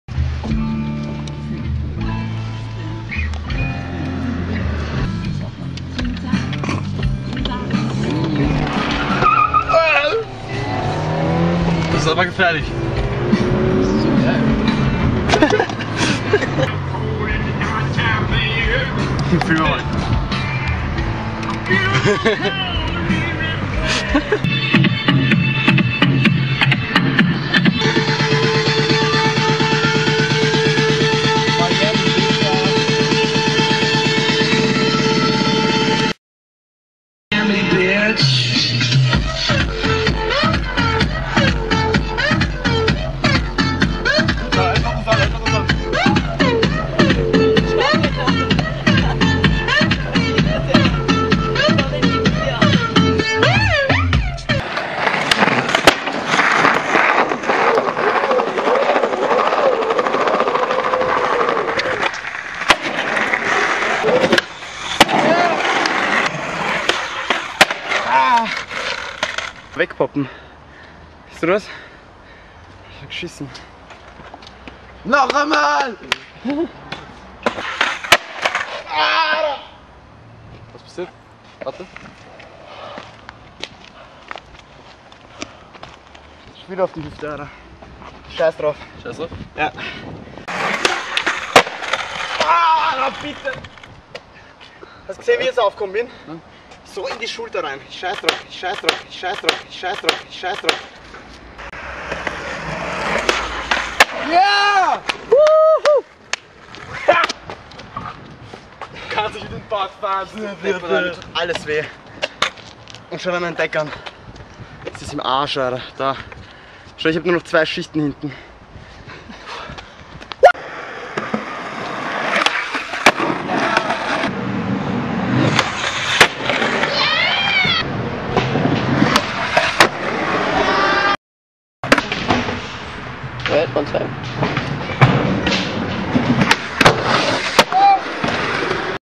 Sunday skate session
A Sunday in Vienna skating with Ramin Valipour and Nicolai Carstensen.